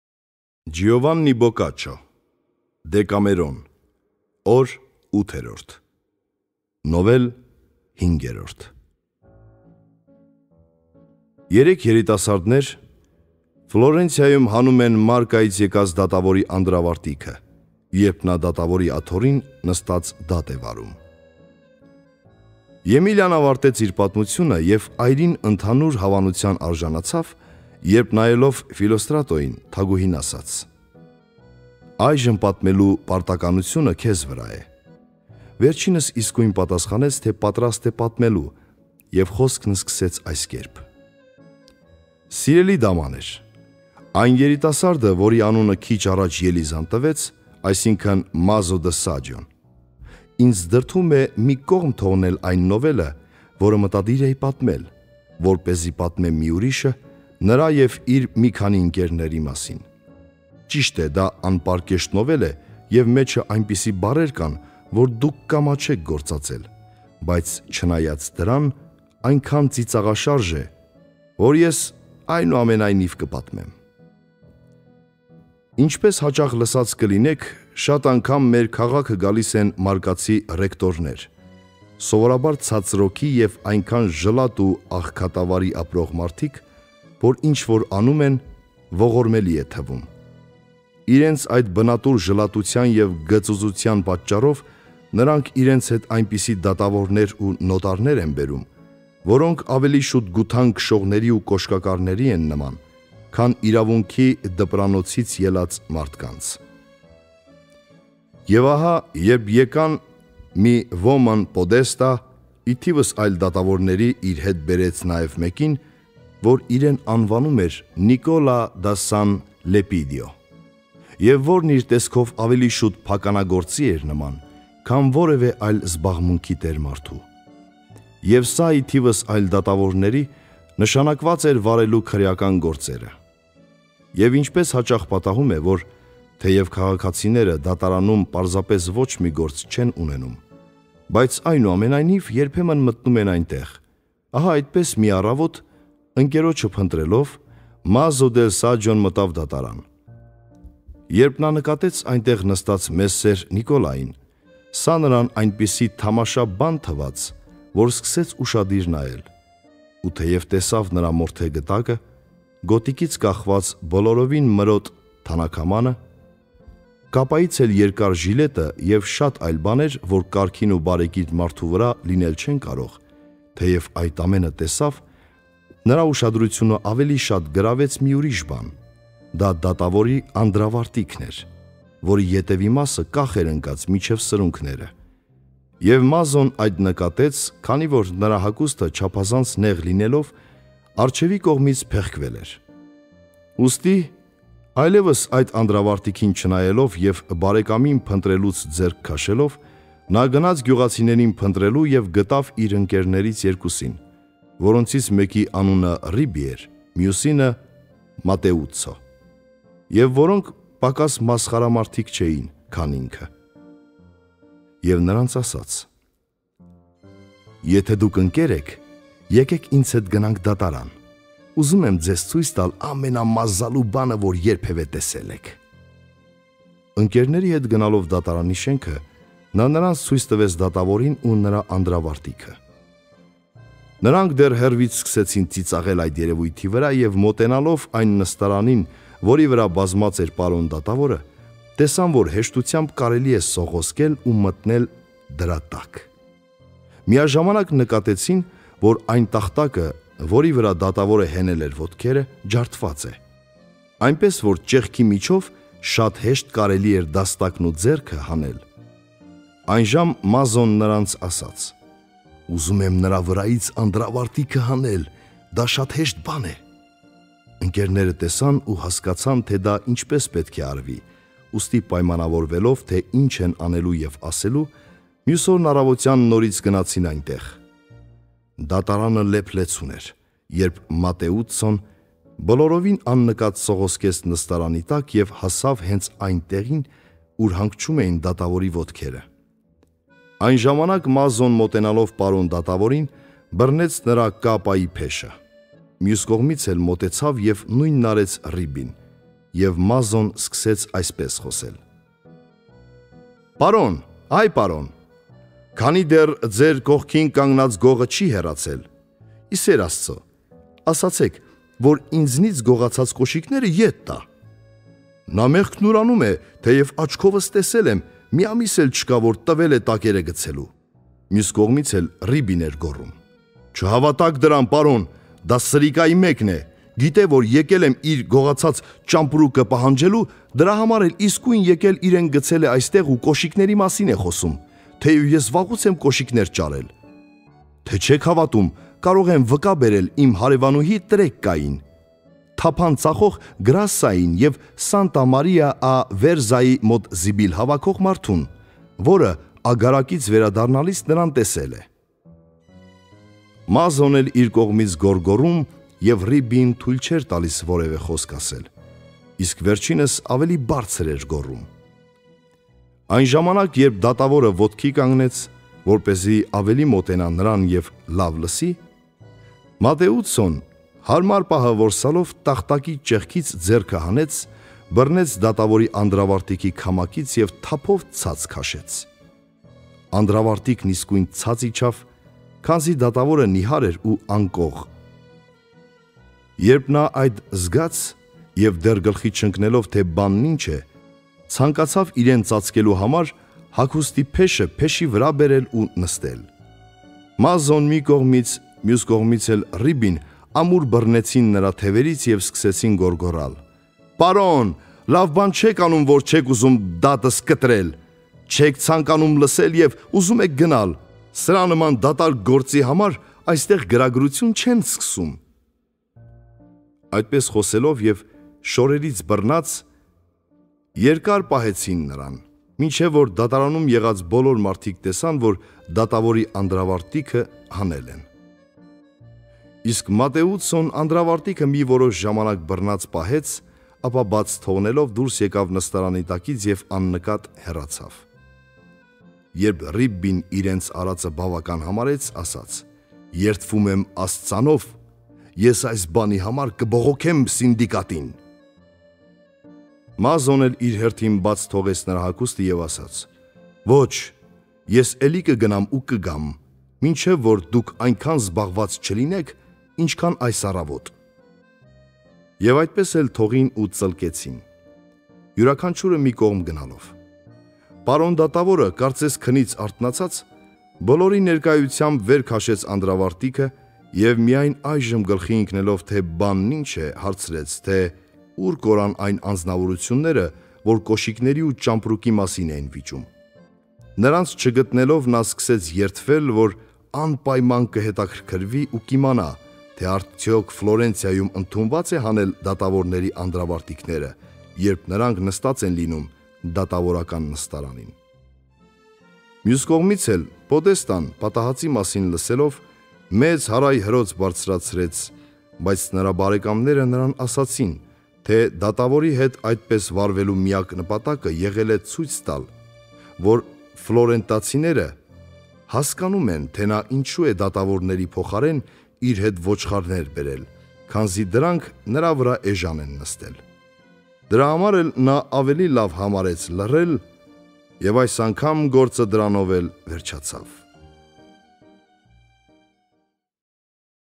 ԴԵԿԱՄԵՐՈՆ - 8/5
Կարդում է Արթուր Մուսայելյանը

Ջովանի Բոկաչչոն ծնվել է 1313 թ. Հունիս-Հուլիս ամիսներին Ֆլորենցիայում:
Դեկամերոնը գրել է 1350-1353 թթ., հրատարակվել է 1470 թ.:
Ջովանի Բոկաչչոն մահացել է 1375 թ. Դեկտեմբերի 21-ին։
Դեկամերոնը հայերեն թարգմանվել և առաջին անգամ հրատարակվել է 1876 թվականին Զմյուռնիայում։